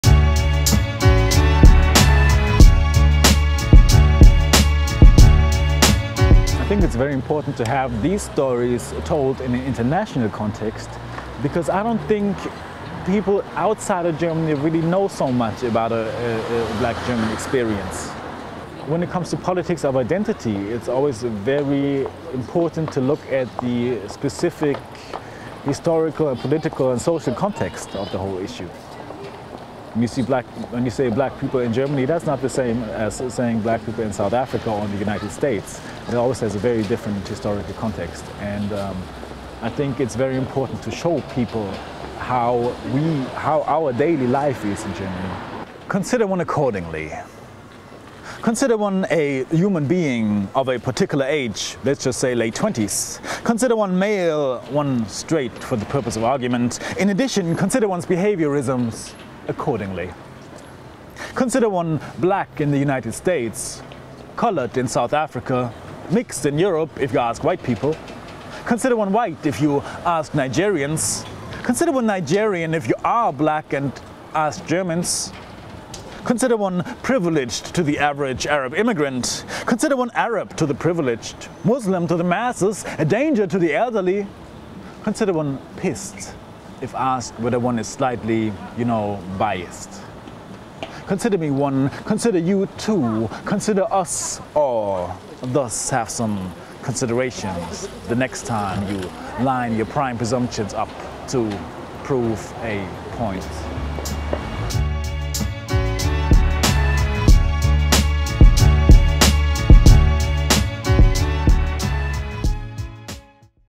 Witnessed II
Witnessed is a book series written in English by Black authors, who live or have lived in Germany, and is edited by Sharon Dodua Otoo.